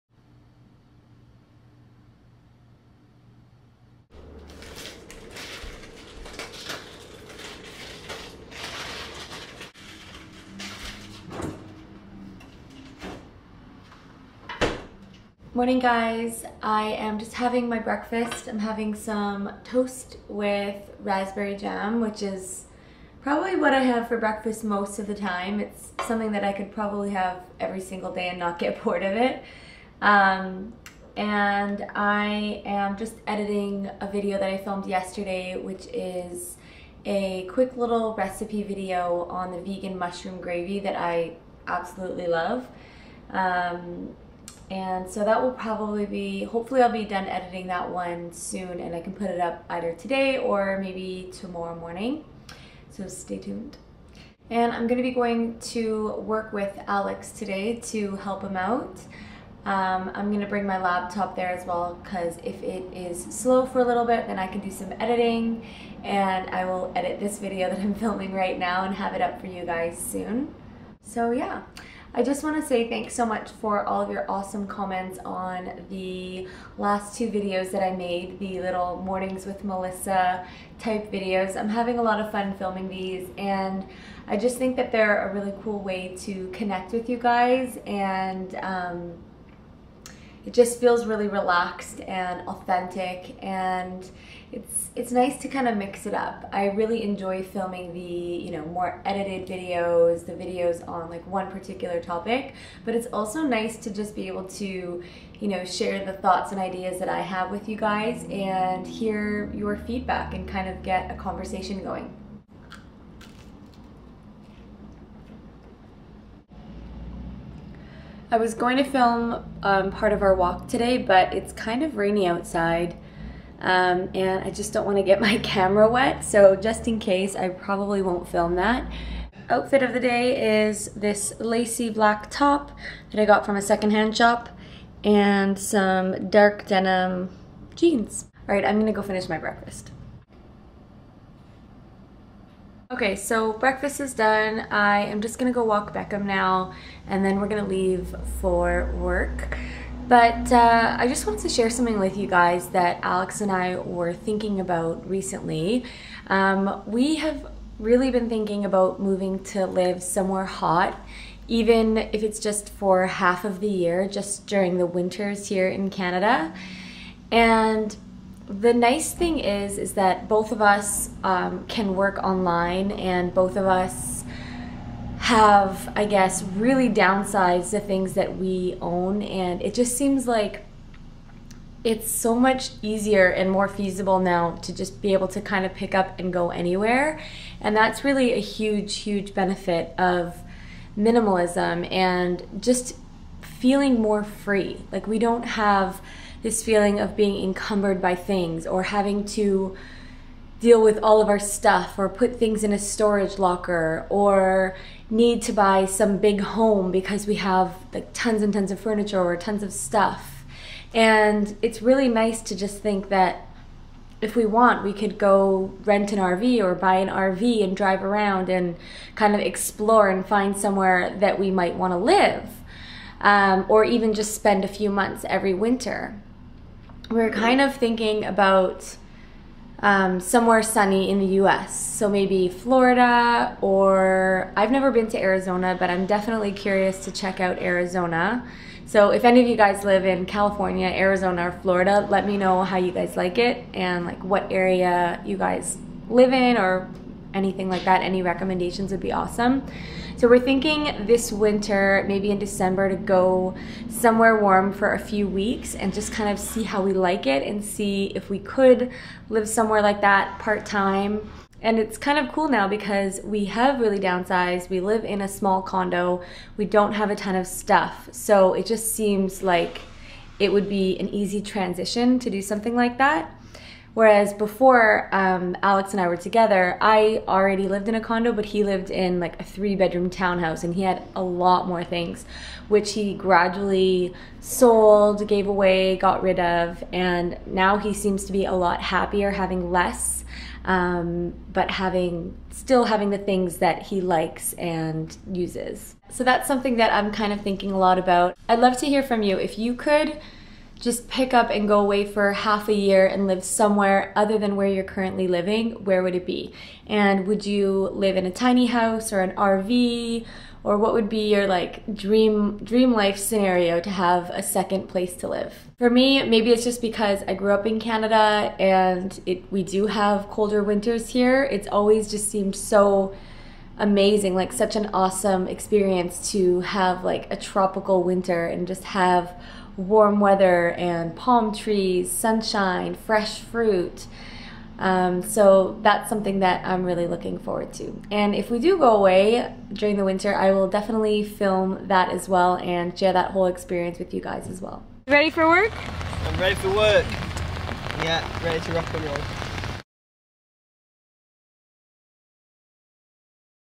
"Mornings with Melissa" Ep. 3 - Minimalism, Freedom to travel, Buying an RV?
Good Morning! :)

Quote of the Day: "A mind that is stretched by a new experience can never go back to its old dimensions."
- Oliver Wendell Holmes, Jr.

Camera: Nikon s9900

xx Melissa